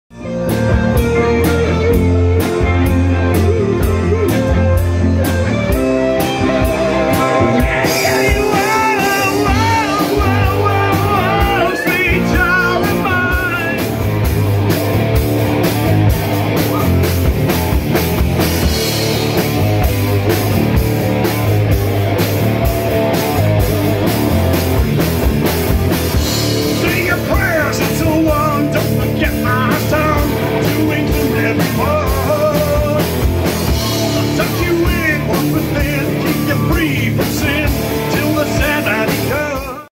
Harbour Lights, Dec 2021
Some clips from the Fitliken gig at the Harbour Lights, Peterhead.